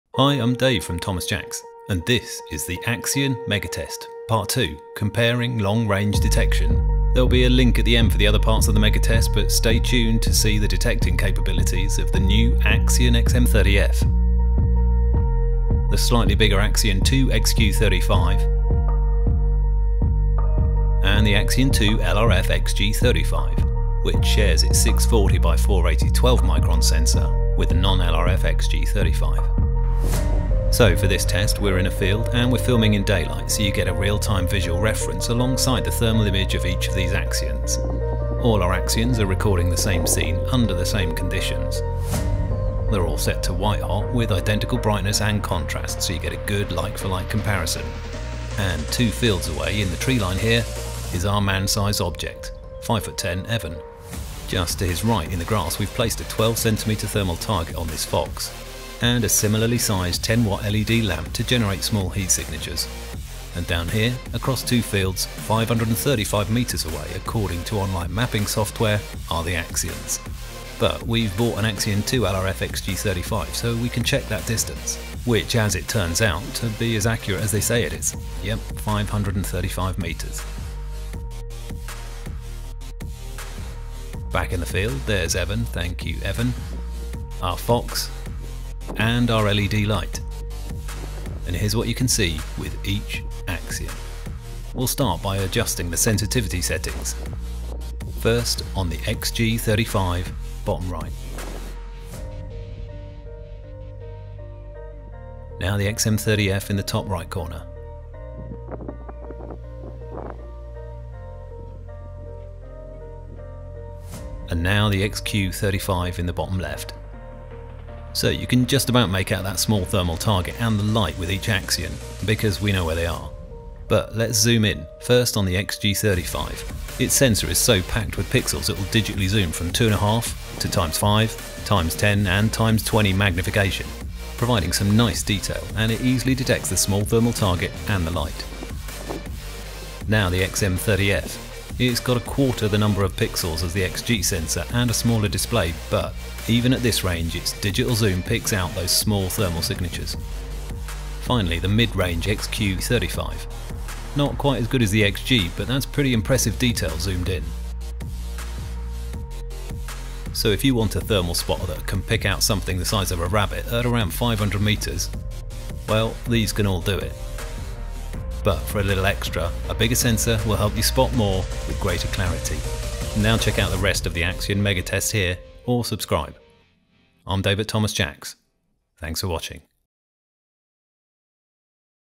XM30F vs XQ35 vs XG35. Part 2: Pulsar Axion LONG RANGE DETECTION mega test.
We wanted show you the difference between the new range of Pulsar Axion handheld thermal monoculars, to help you choose the right one for your needs and your budget.

In this second test you’ll see unedited footage from the Axion XM30F, alongside the Axion 2 XQ35 and Axion 2 XG35 at long range – over 500m. So you can compare the image quality that helps you detect people or animals the size of a rabbit across a couple of fields.

You’ll see a number of different warm objects, and we’ll explain what’s going on for each Axion while we adjust some of their settings and test out their digital zoom.